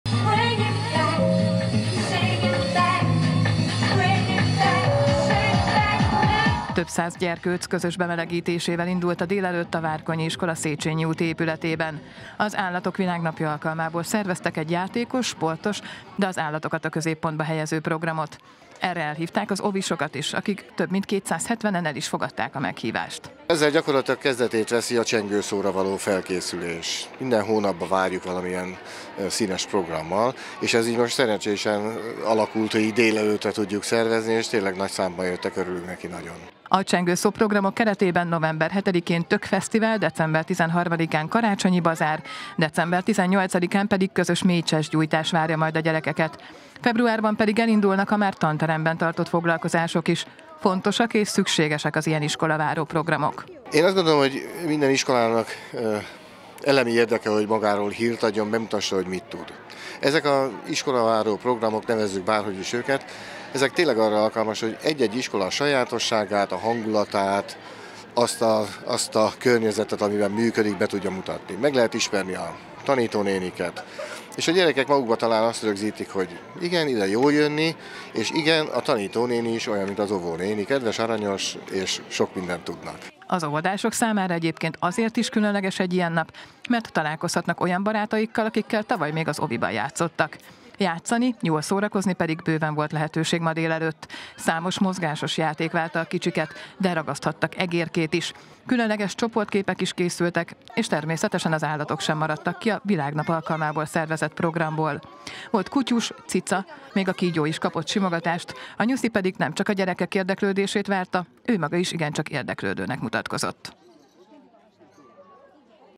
Állatok Világnapja - sok mozgással
Az Állatok Világnapja alkalmából hívta játékos délelőttre az óvodásokat a Várkonyi iskola. Természetesen azonban az iskola legkisebb tanulói is részt vettek a programokon, vagyis ők is játszhattak, versenyezhettek és persze ők is gyönyörködhettek a néhány órára az udvarba költöző állatokban.